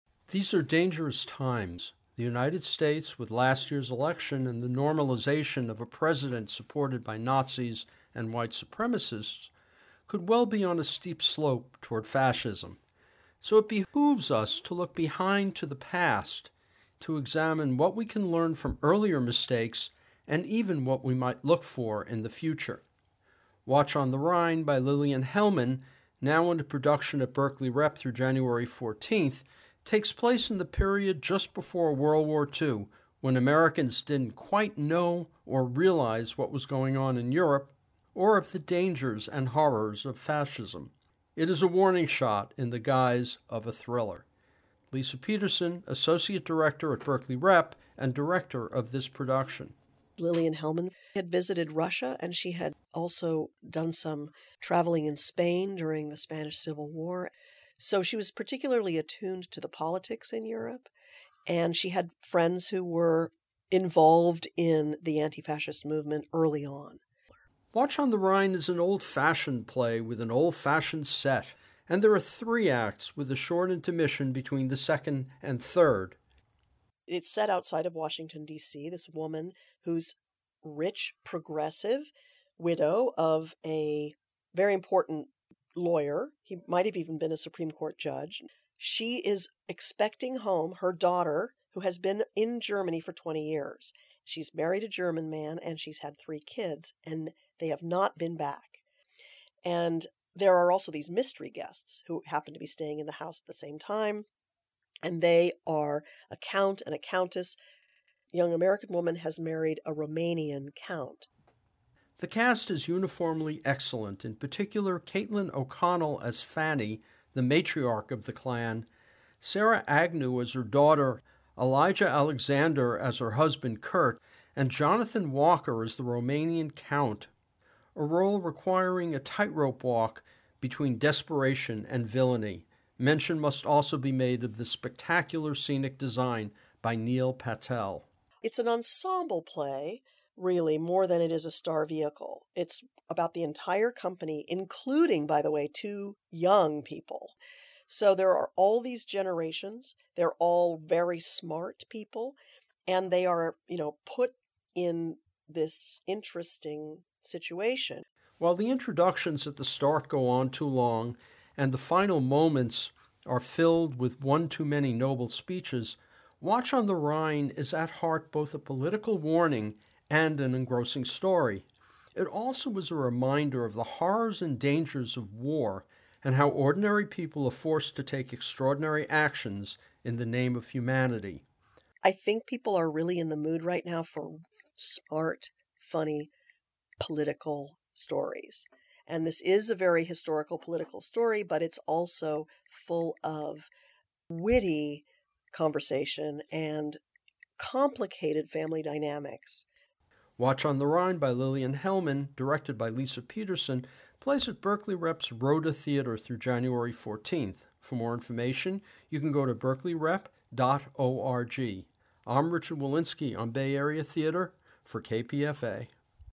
Review: Watch on the Rhine at Berkeley Rep
KPFA theatre critic Richard Wolinsky reviews “Watch on the Rhine” by Lillian Hellman, at Berkeley Rep through January 14, 2017.

Berkeley Rep website

The post Review: Watch on the Rhine at Berkeley Rep appeared first on KPFA